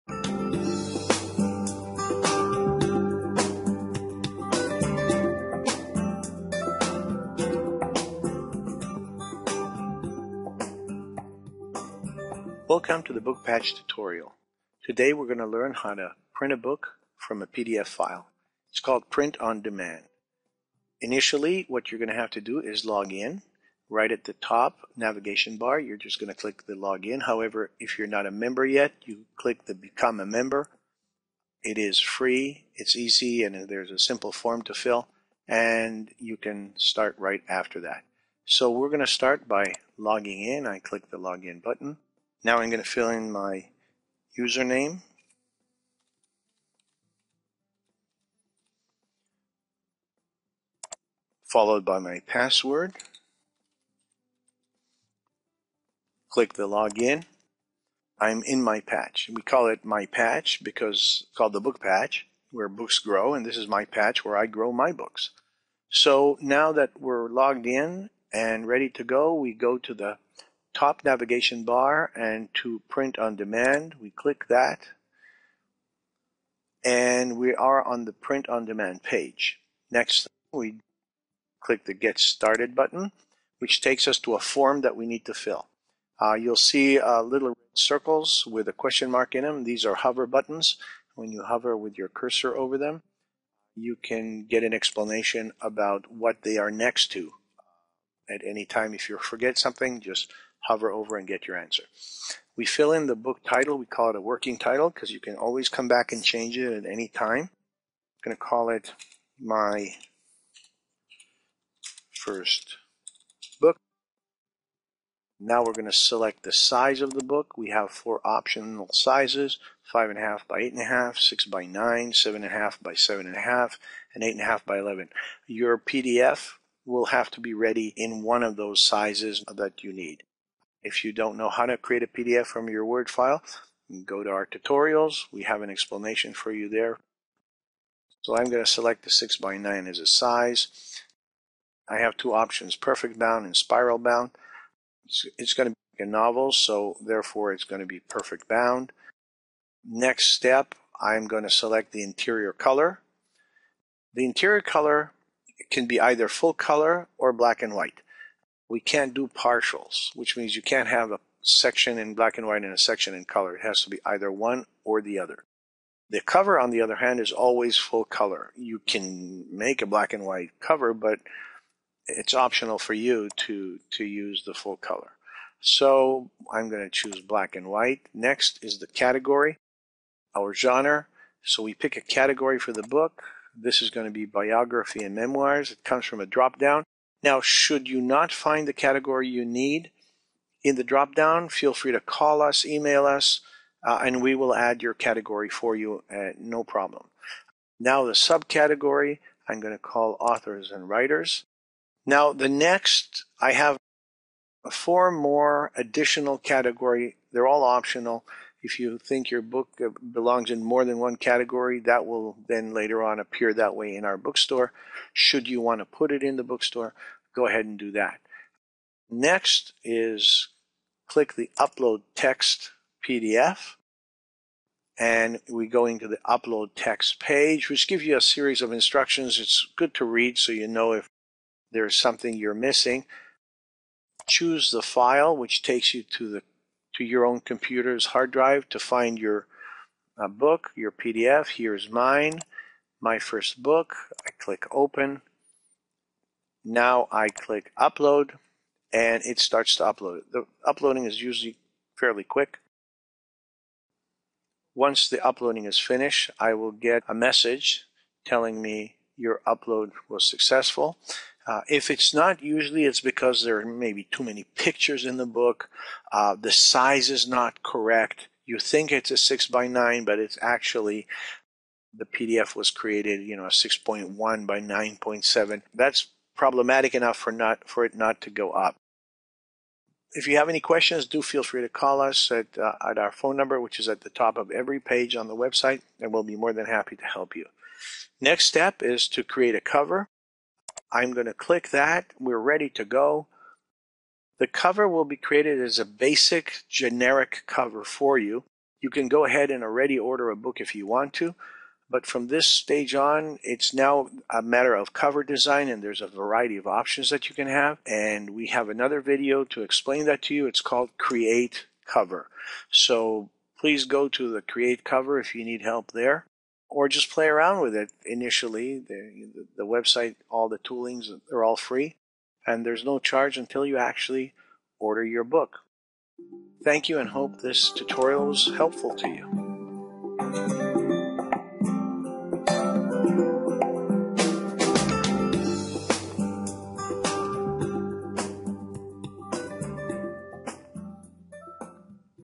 Creating a book from a PDF on TheBookPatch.mp4
How to start printing a book on demand using the TheBookPatch.com